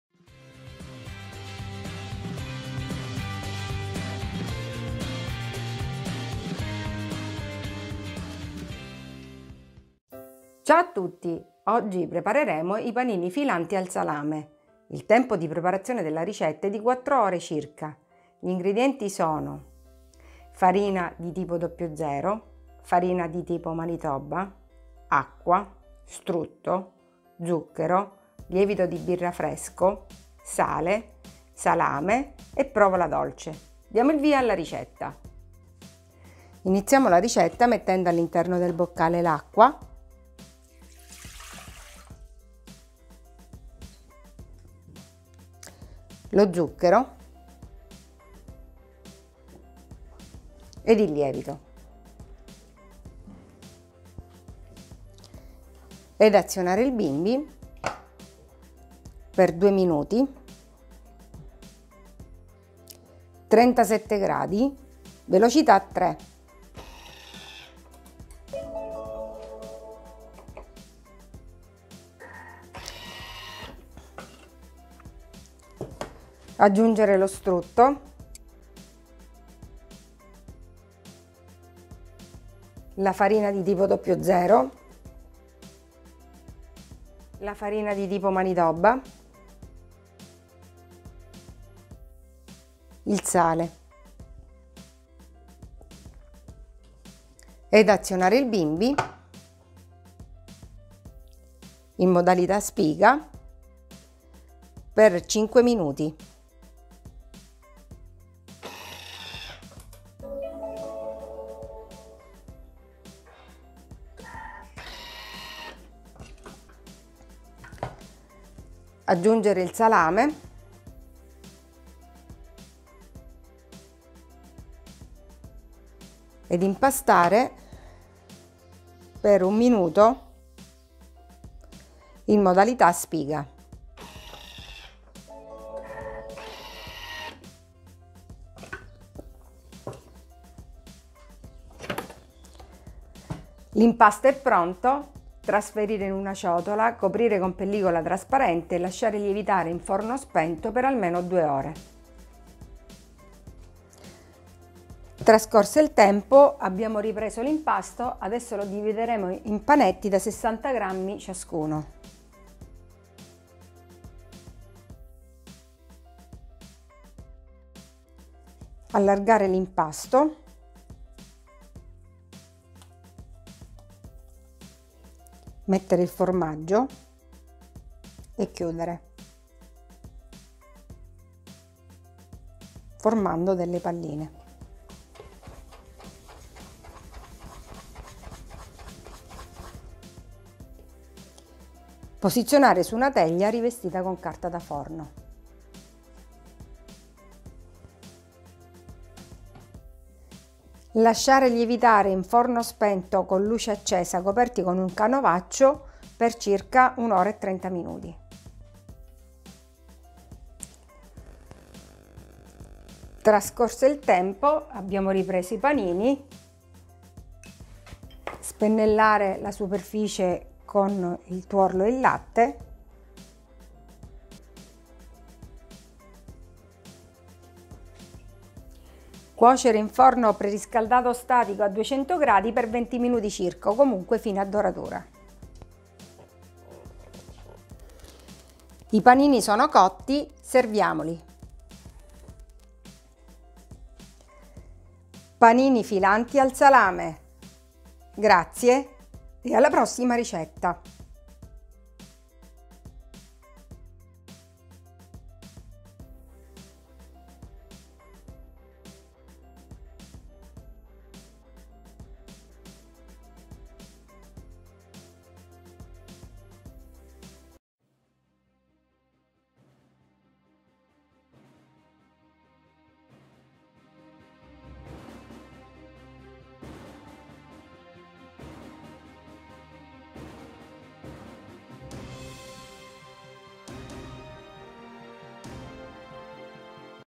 Panini filanti al salame per bimby TM6 TM5 TM31 TM21
Ingredienti:
300 g di farina di tipo "00"
300 g di farina di tipo manitoba
280 g di acqua
80 g di strutto
40 g di zucchero
11 g di lievito di birra fresco
15 g di sale
150 g di salame tagliato a pezzetti
180 g di provola dolce tagliata a cubetti
tuorlo e latte q.b. per spennellare